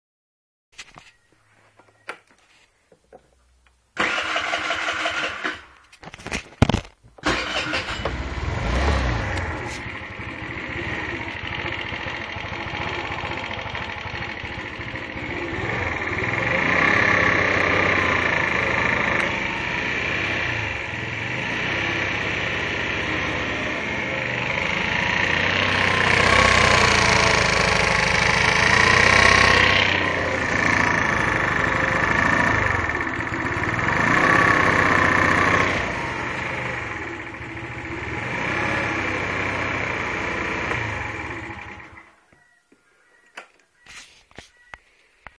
Problem 4t 50cm
Wie ktoś co może być przyczyną takiej pracy silnika?
Proszę o odpowiedź w komentarzach :D